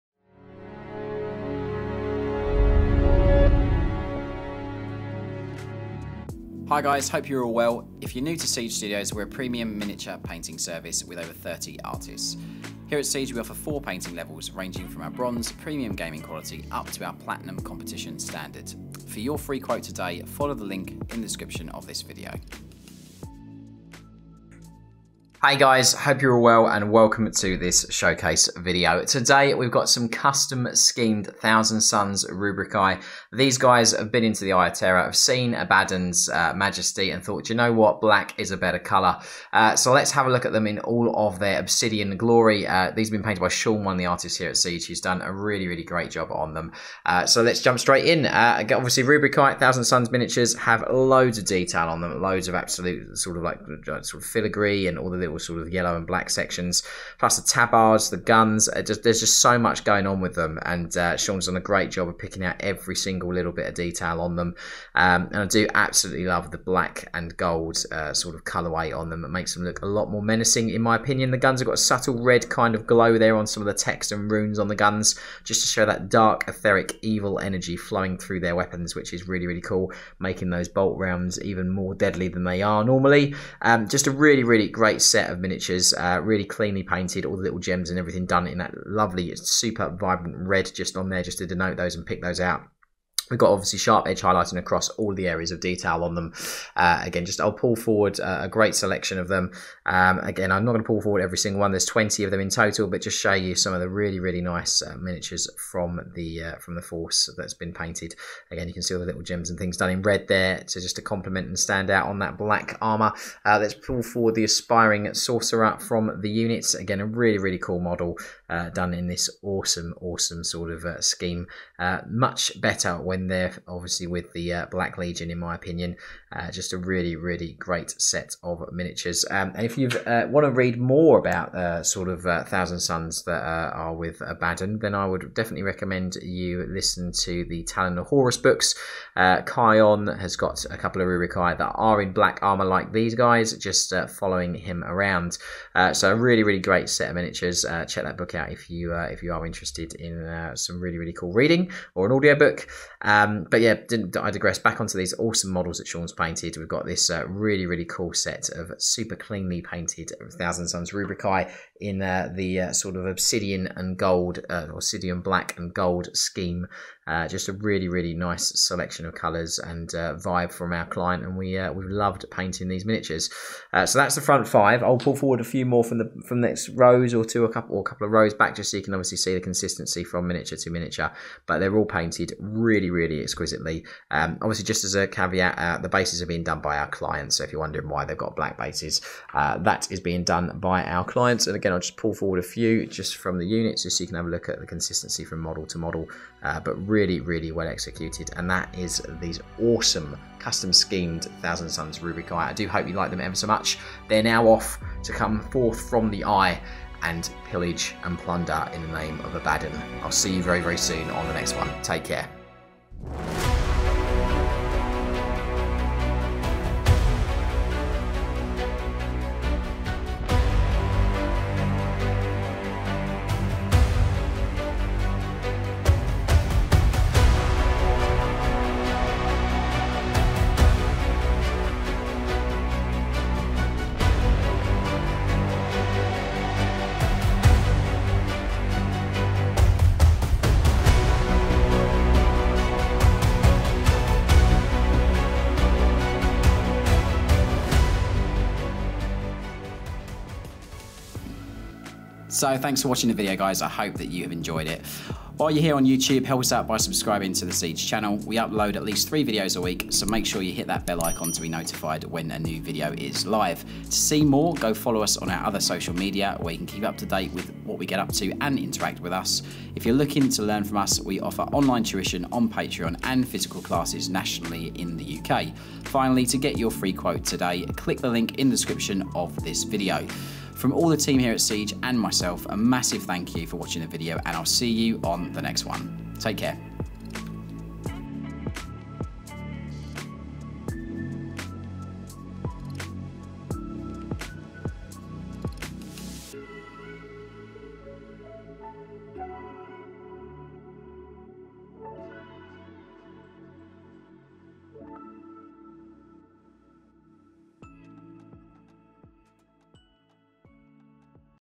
TZEENTCH BLACK LEGION! Sons of the Cylops Thousand Sons Warhammer 40k Painting Showcase
For our showcase today we have this great Sons of the Cyclops Thousand Sons Warhammer 40k commission including 20 Rubric Marines completed to our Bronze painting level, with no basing.

00:00 INTRO
00:24 OVERVIEW
00:48 RUBRIC MARINES
03:32 DISPLAY SHOWCASE
04:11 OUTRO

Thanks for joining us and we'll see you on Wednesday!